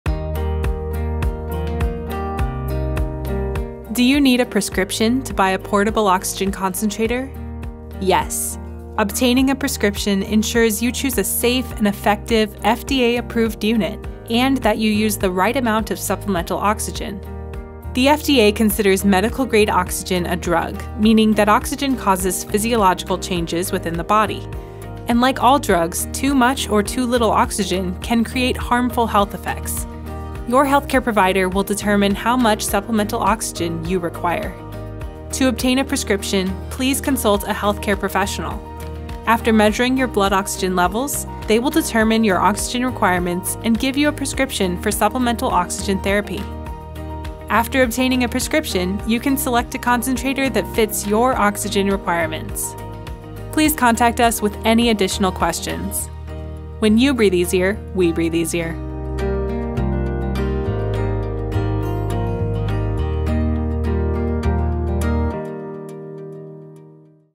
Do You Need a Prescription to Buy a Portable Oxygen Concentrator?
Yes. Obtaining a prescription ensures you choose a safe and effective FDA-approved unit and that you use the right amount of supplemental oxygen.